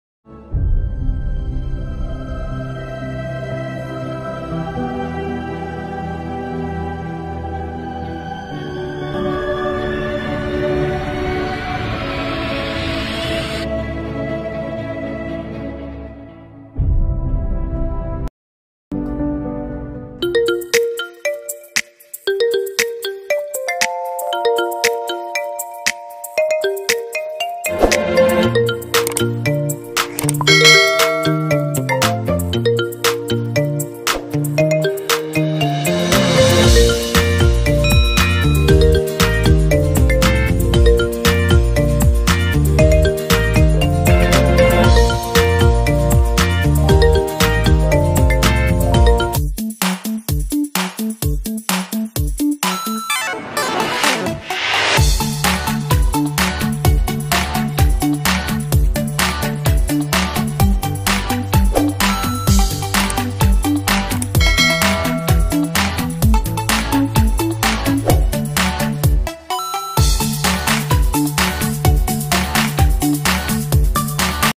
Brother and sister play/අයියගේයි මල්ලිගේයි සේල්ලම බලන්න /Dog/play/sinhala
The two puppies play on the playground and then go to sea to play